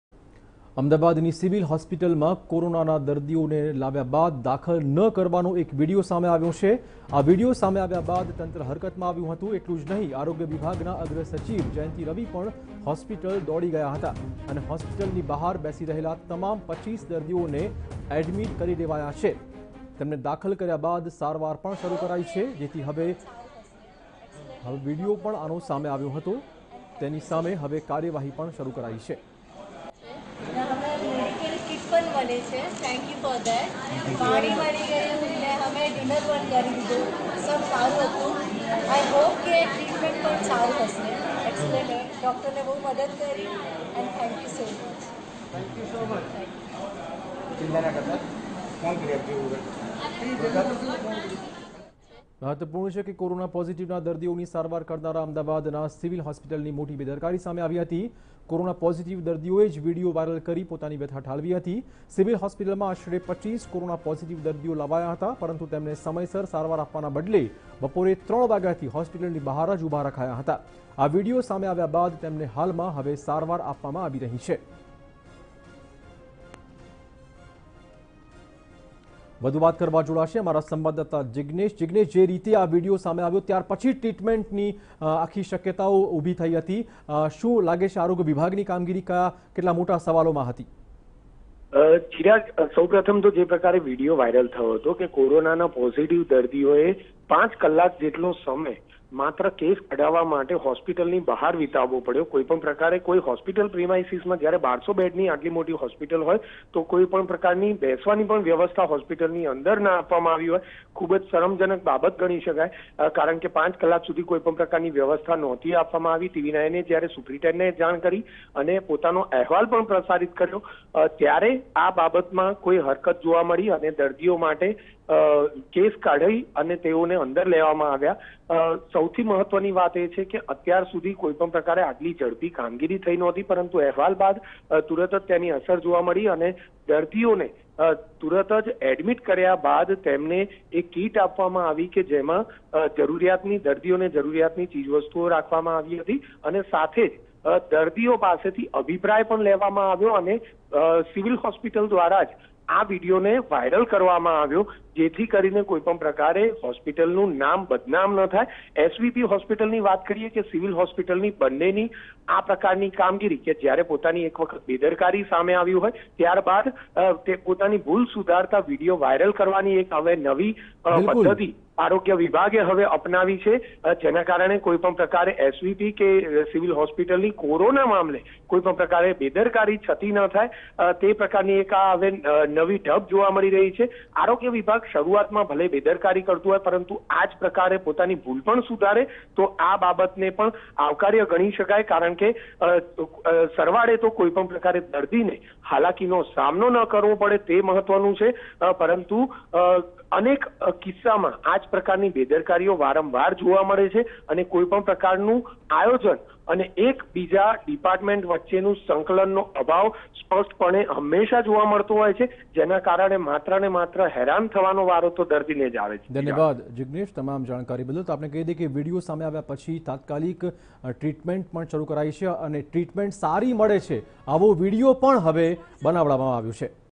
Case of COVID19 patients waiting for admission in Civil hosp.;All of them admitted after news report
Ahmedabad: Case of COVID-19 patients waiting for admission in Civil hospital; All of them admitted after news report| TV9